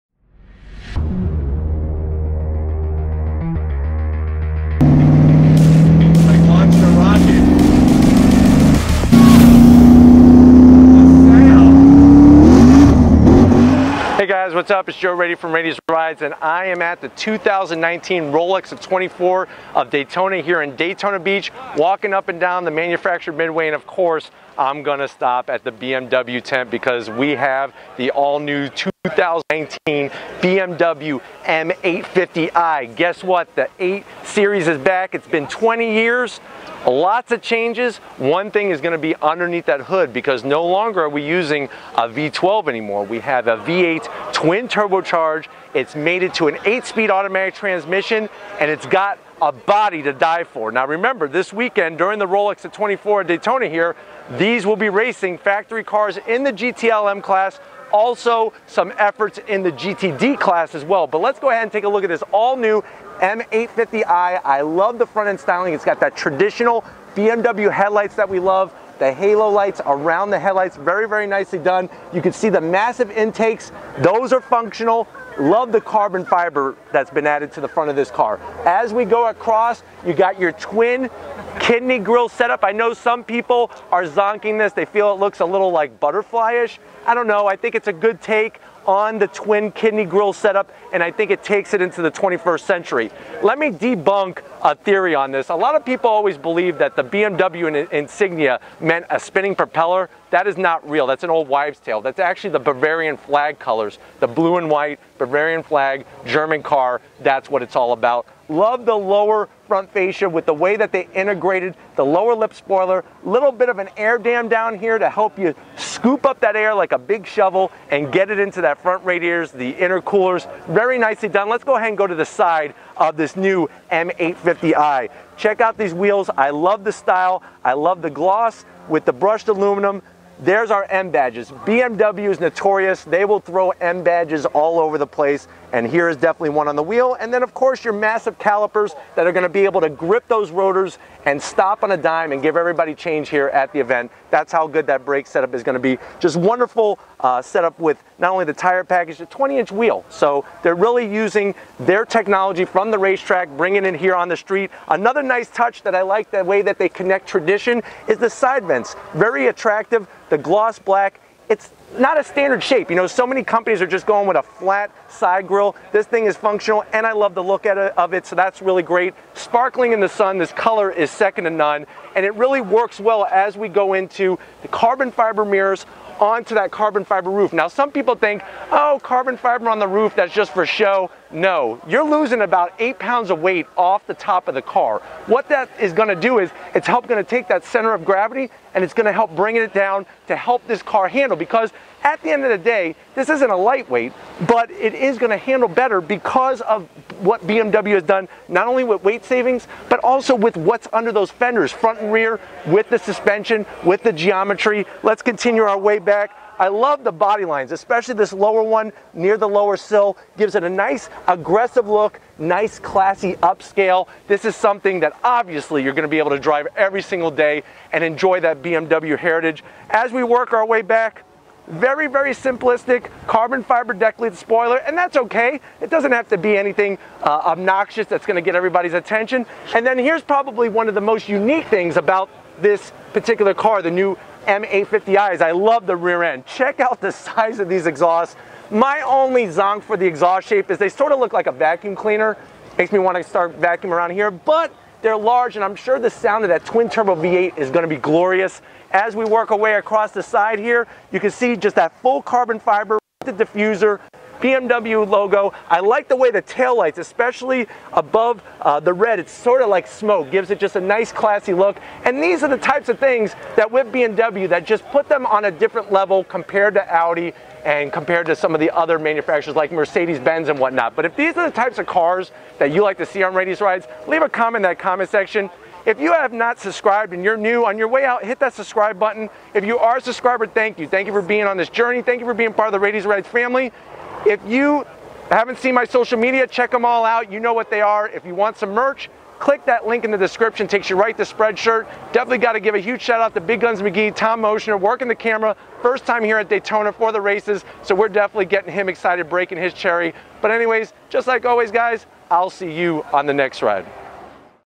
Is the 2019 BMW M850i deliver on what you waited for?
The ALL NEW 2019 BMW M850i xDrive is finally heere. Under the hood you will no longer find a V12 but a 4.4L V8. BMW developed an exterior that would cut through the air and have all the distinctive BMW attributes, but is it enough? Check out the details and see for yourself.